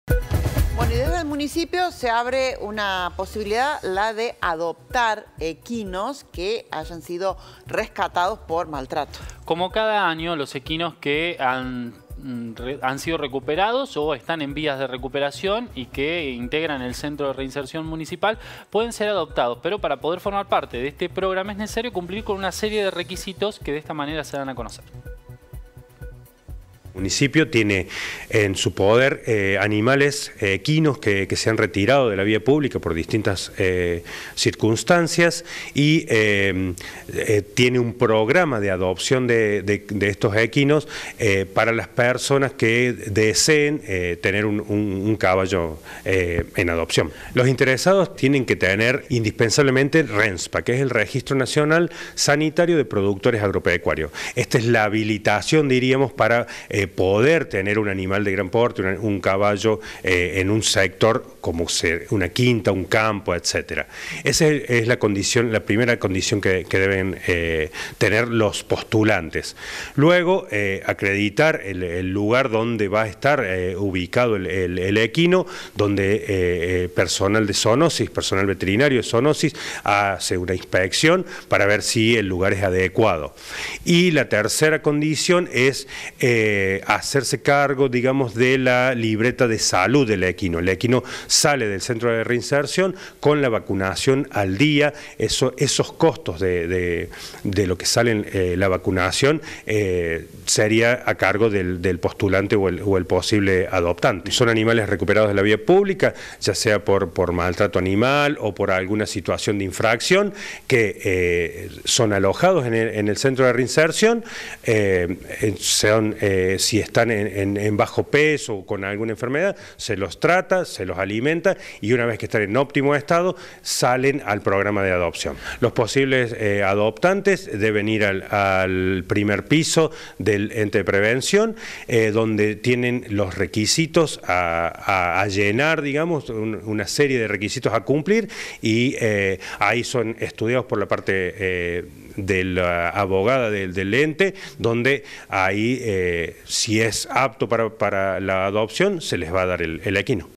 La municipalidad dio a conocer los requisitos para adoptar equinos
La municipalidad dio a conocer los requisitos para adoptar
equinos que fueron rescatados y están en el centro de reinserción

PAULO RIZOS - Director de Zoonosis